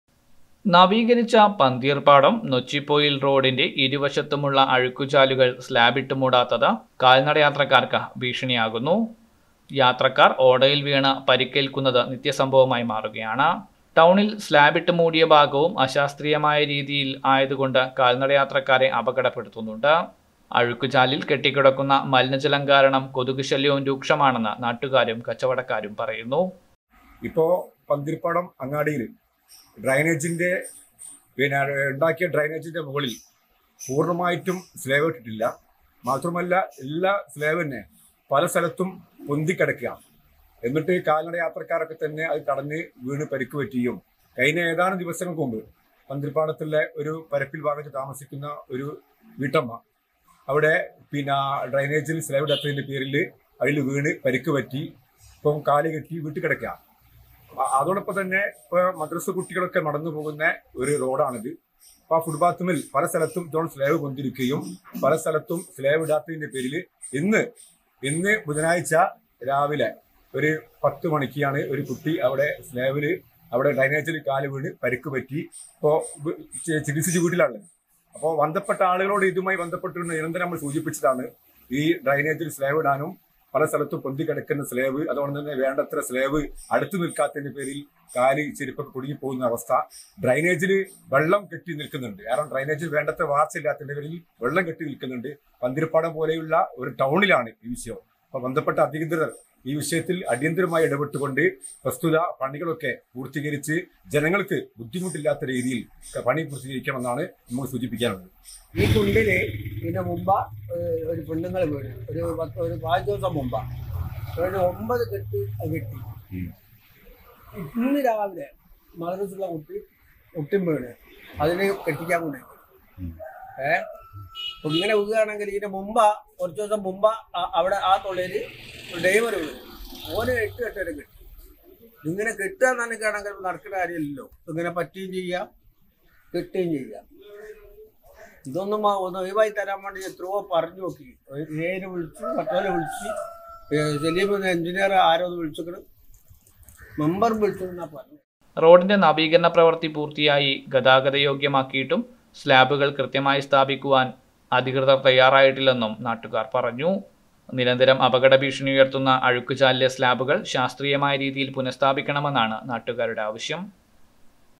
അഴുക്കുചാൽ സ്ലാബിട്ട് മൂടാത്തത് കാൽനടയാത്രക്കാർക്ക് ഭീഷണിയാകുന്നു
നവീകരിച്ച പന്തീർപ്പാടം നെച്ചിപൊയിൽ റോഡിന്റെ ഇരുവശത്തുമുള്ള അഴുക്കുചാൽ സ്ലാബിട്ട് മൂടാത്തത് കാൽനടയാത്രക്കാർക്ക് ഭീഷണിയാകുന്നു. യാത്രക്കാർ ഓടയിൽ വീണ പരിക്കേൽക്കുന്നത് നിത്യസംഭവമാണ്